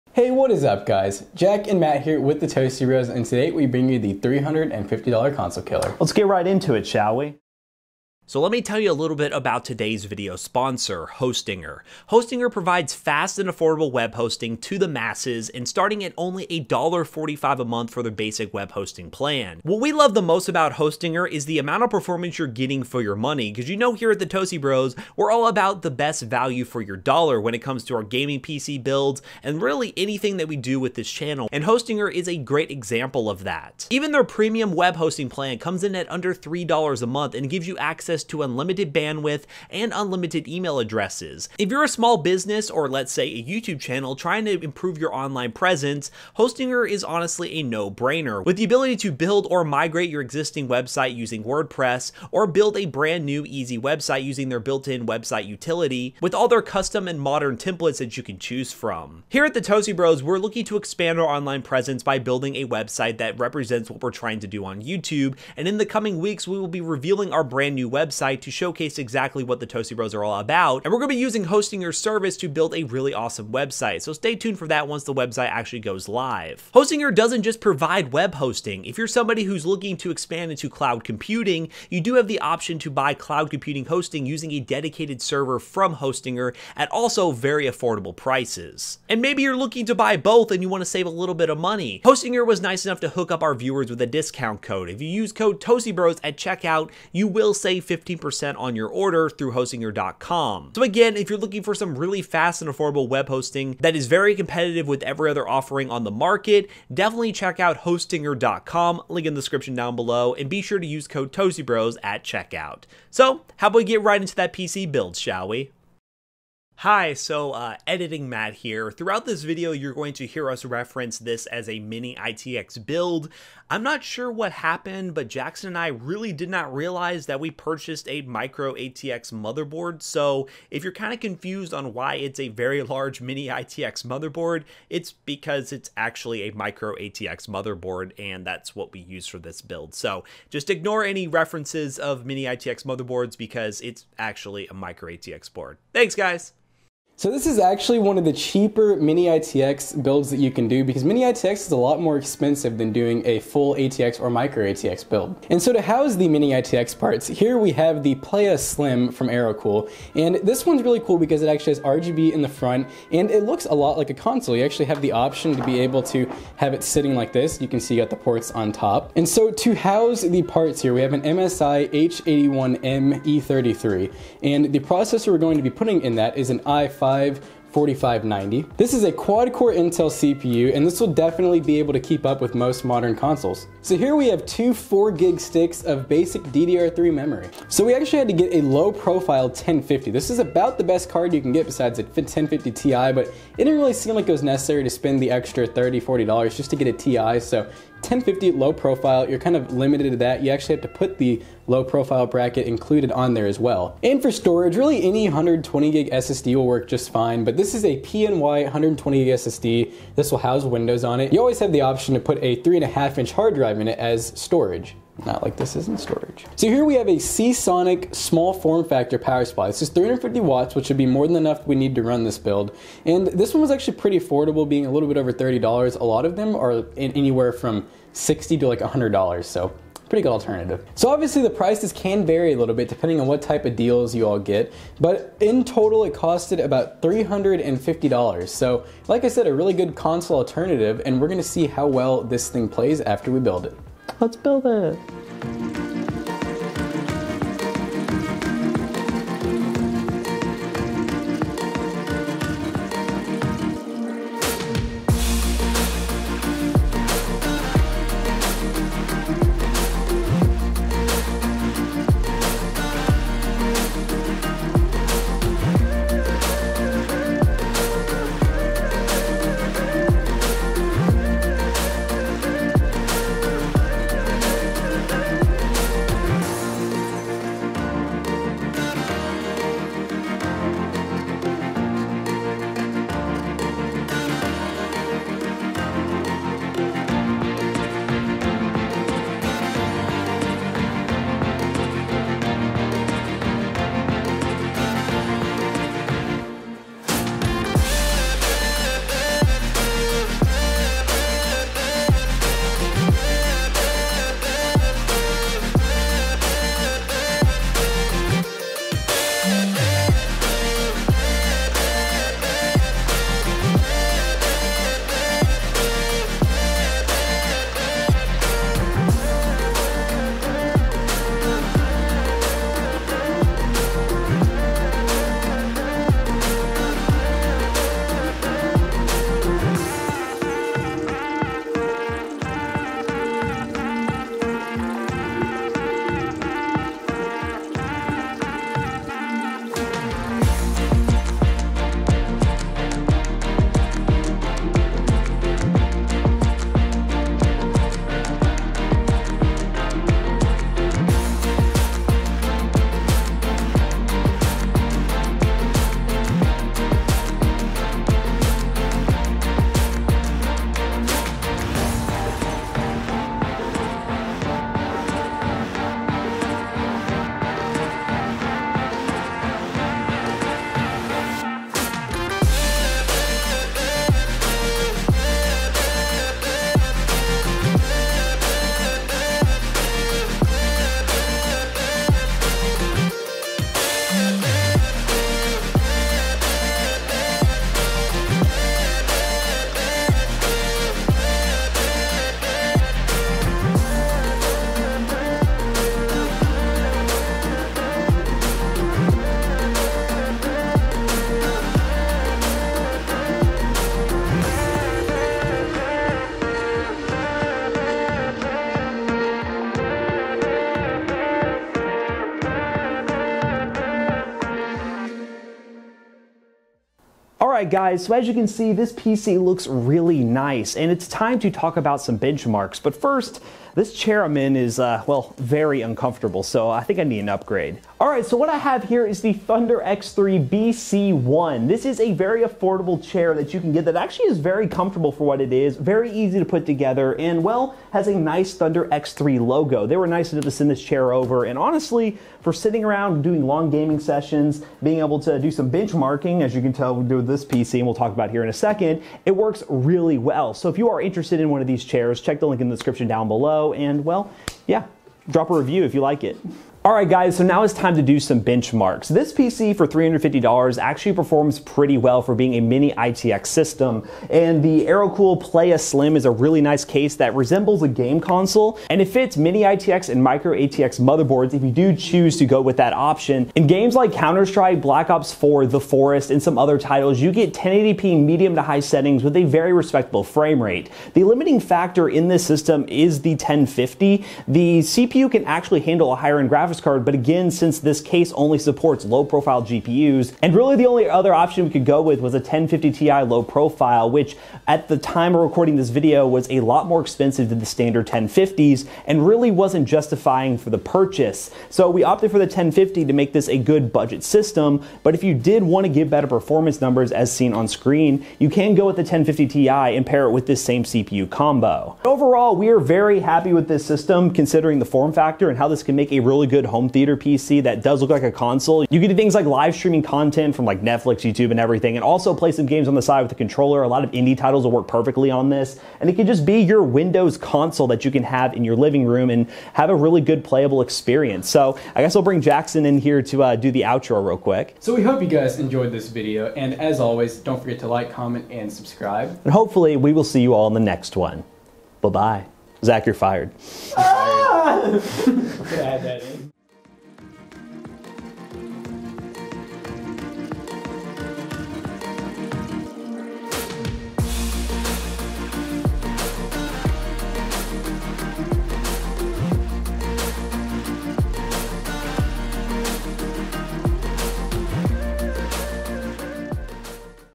$350 Gaming PC Build | Console Killer?
Today we are taking on the task of building a $350 gaming pc inside the Aerocool Playa Slim, a "console-like" case that only supports low profile GPU's. Is it worth considering for your next build?

✊ Parts List ✊

- i5 4590
- MSI H81M-E33
- GTX 1050 LP
- 8GB DDR3
- 350w SeaSonic SSF PSU
- 120gb SSD